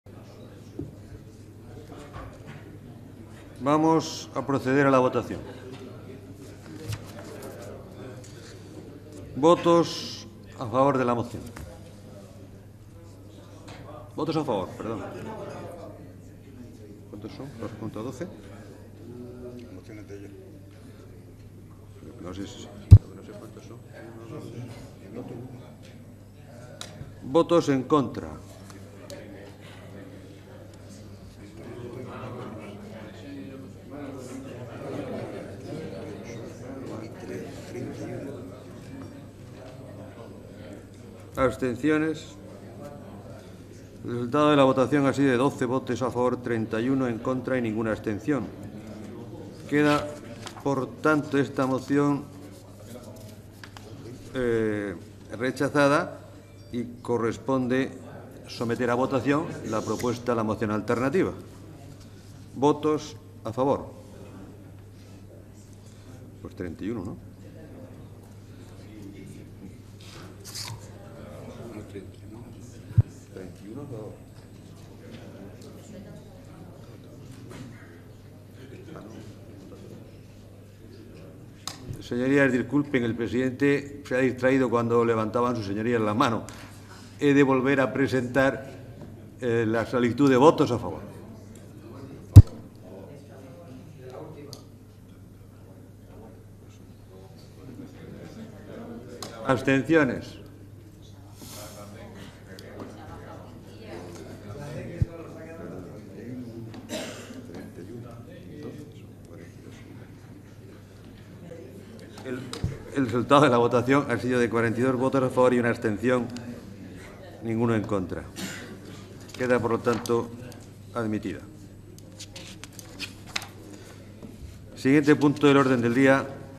Votación.
Debate y votación de cuatro mociones sobre "Vías verdes de la Región", formulada por la diputada del Grupo Parlamentario Popular, Severa González López; "Cumplimiento de las directrices y Plan Territorial de la Comarca del Noroeste, en lo que afecta al paraje de La Rafa de Bullas", del diputado de Izquierda Unida-Verdes Región de Murcia, José Antonio Pujante Diekmann; "Mantenimiento del derecho a la asistencia sanitaria a los ciudadanos de la Región que emigren a otros países y solicitud al Gobierno de la Nación de derogación de dichas medidas", de la diputada del Grupo Parlamentario Socialista, Teresa Rosique Rodríguez y "Medidas de apoyo en el ámbito universitario a personas víctimas de violencia de género", de María Dolores Soler Celdrán, del Grupo Parlamentario Popular.